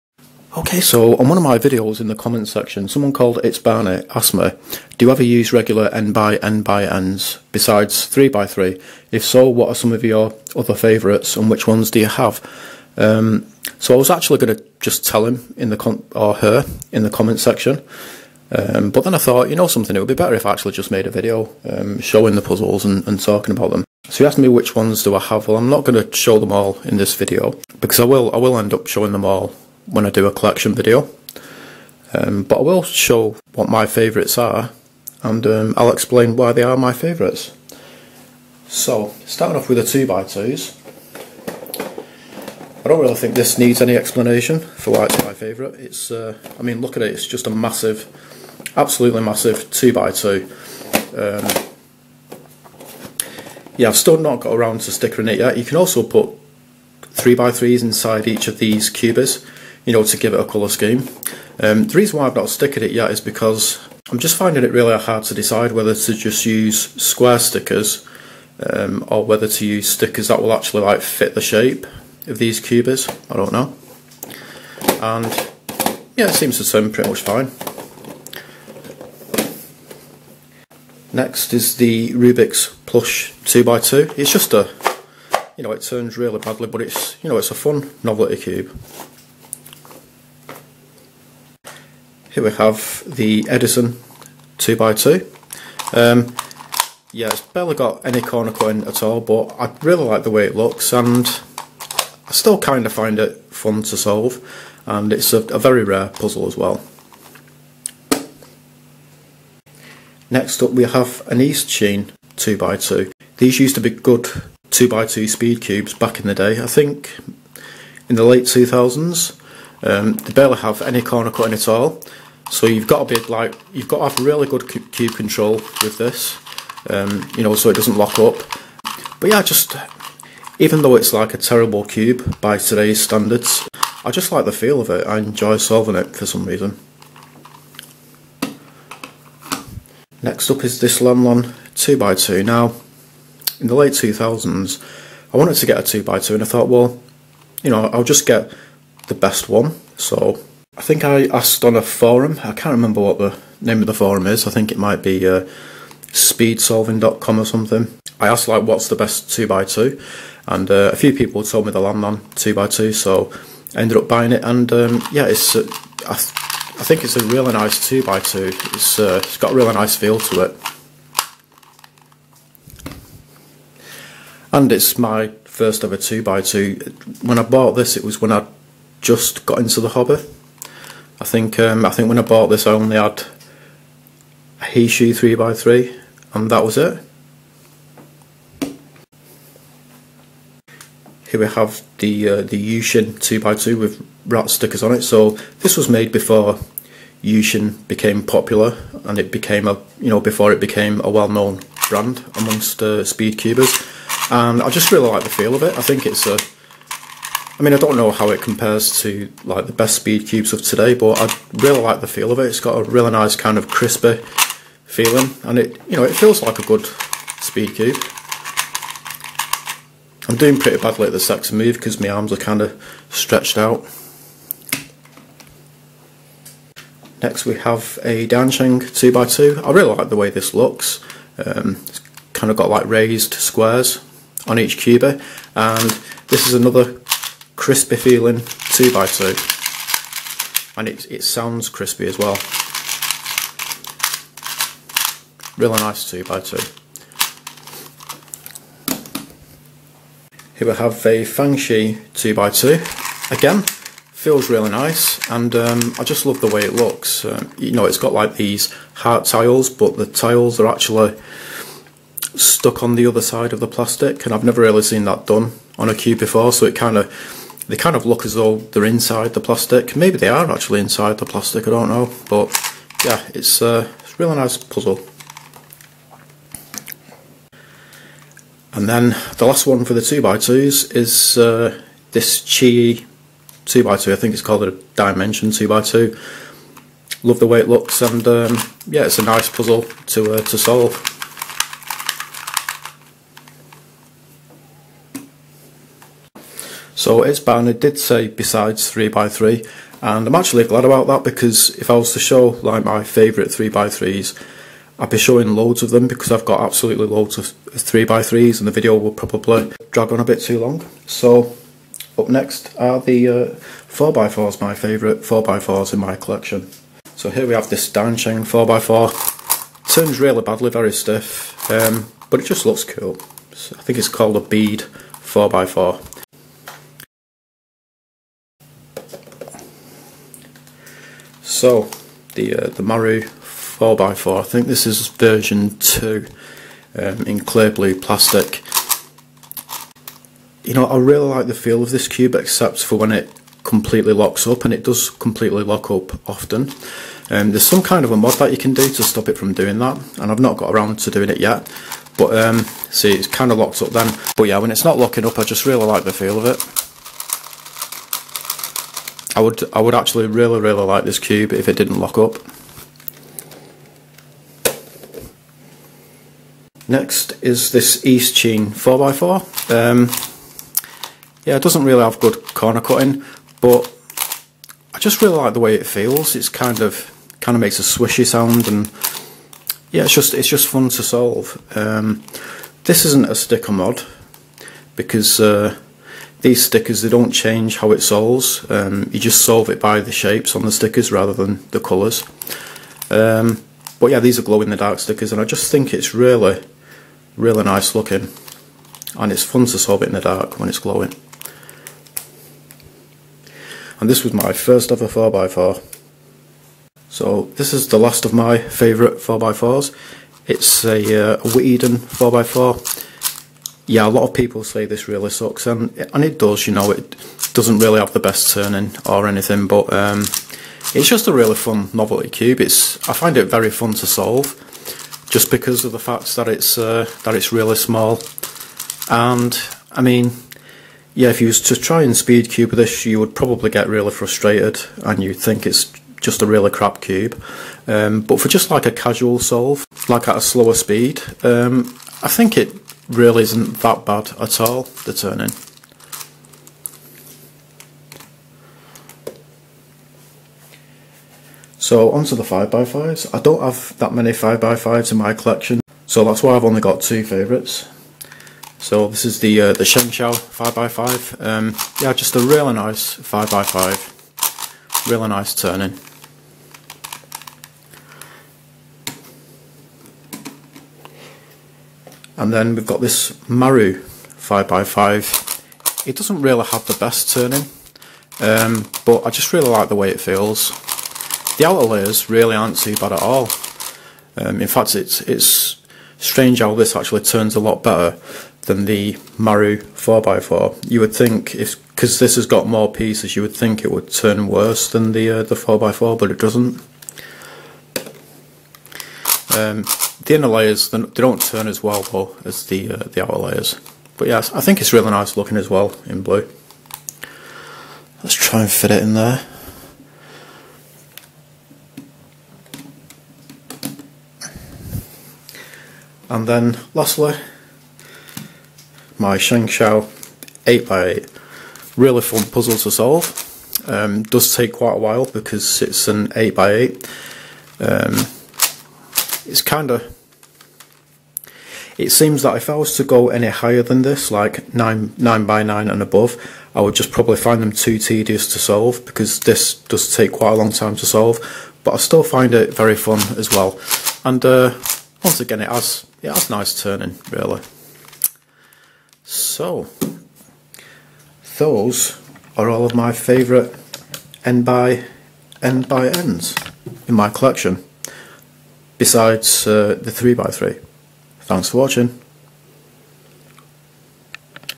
My Favourite NxNxN's In My Collection Excluding 3x3's
Someone asked me what my favorite NxNxN's are in my collection besides 3x3's.